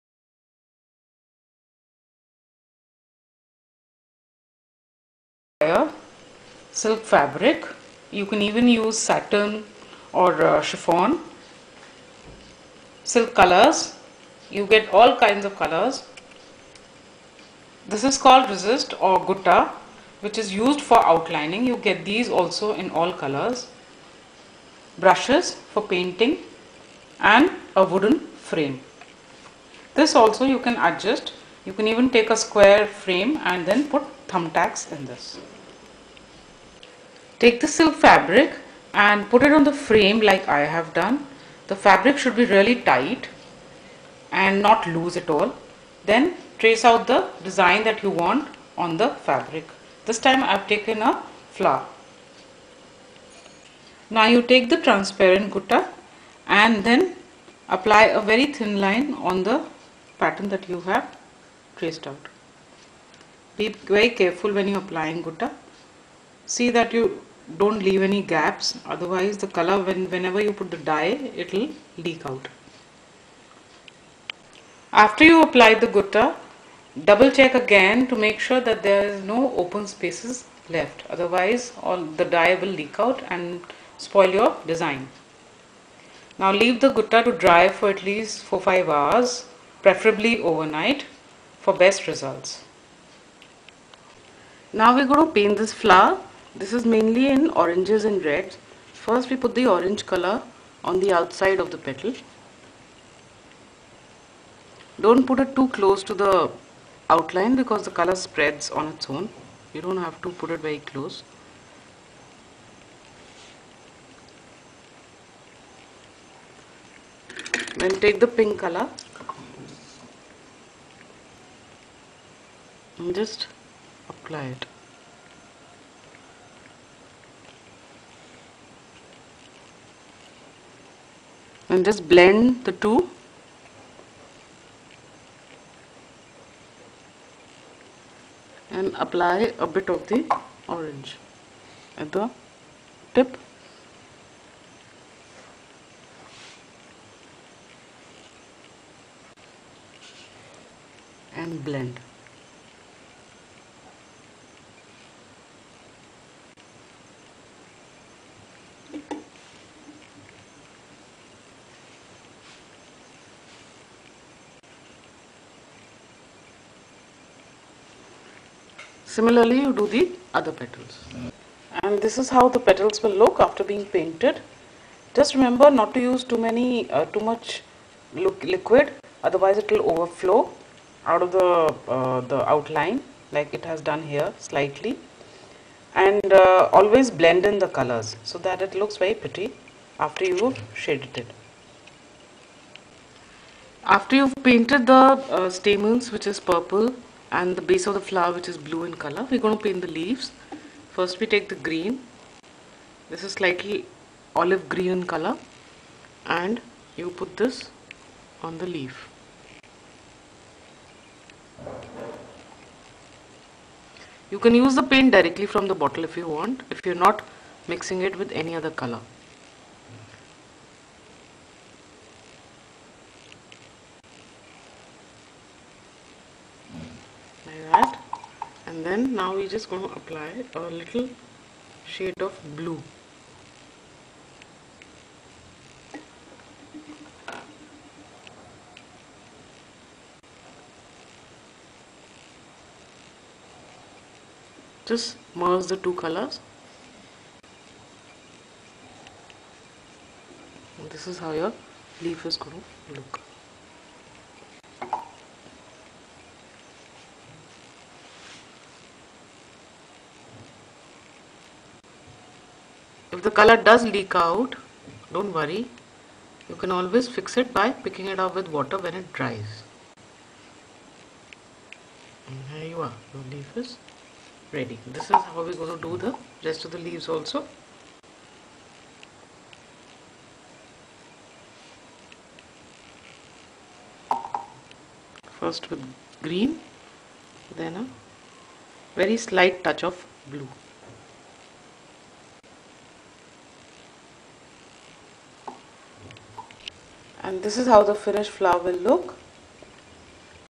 Silk painting
CelebrateArt - Painting on silk originated in Rajasthan in the 17th century. These paintings are artistocratic and individualistic. This video will help us understand that what we buy is a story and not just a product.